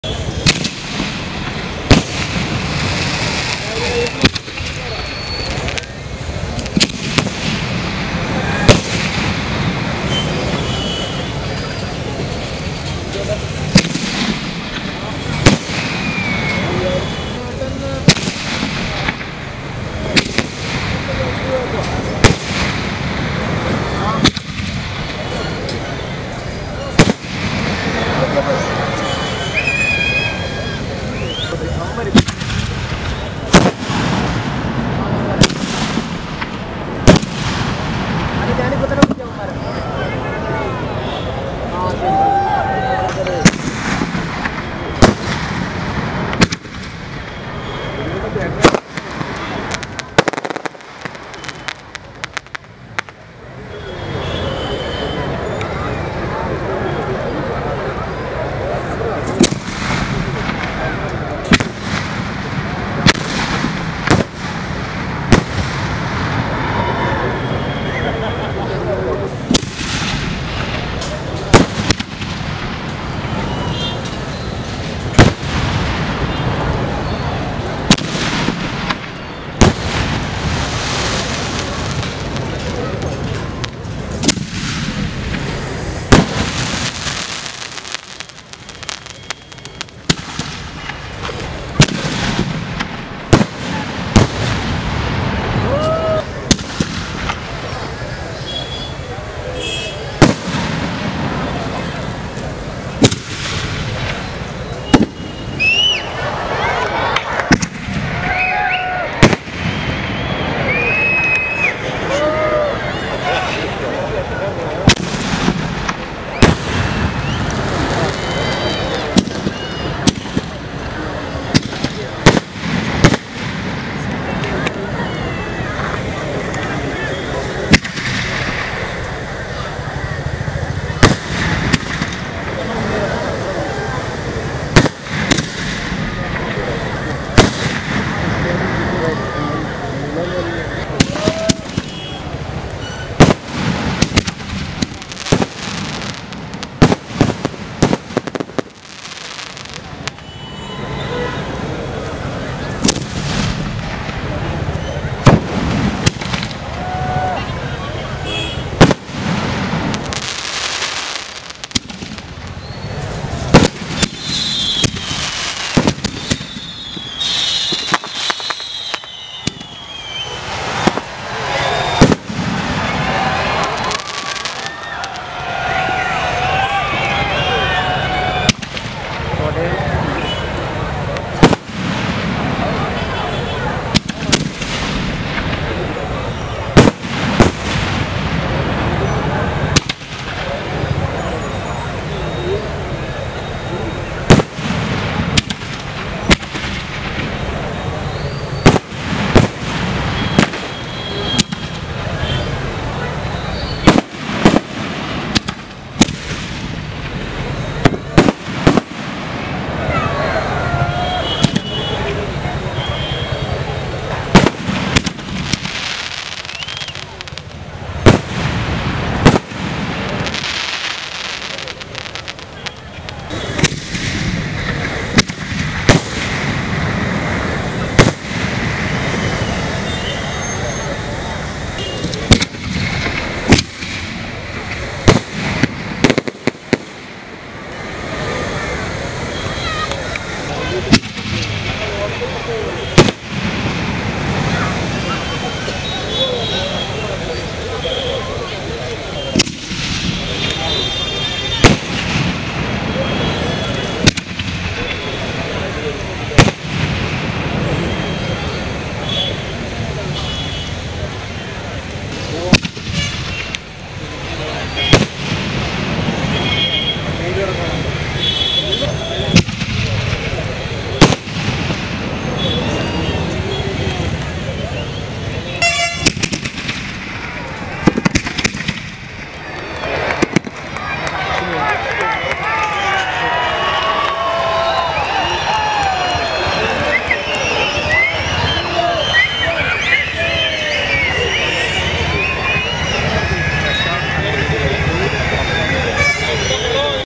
Beautiful Colour Shells -5 | Puthiyakavu 2022 | Vadakkekkepuram
Compilation of a variety of big colour shells fired at Puthiyakavu Bhagavathi Temple, Thripunithura, Kerala.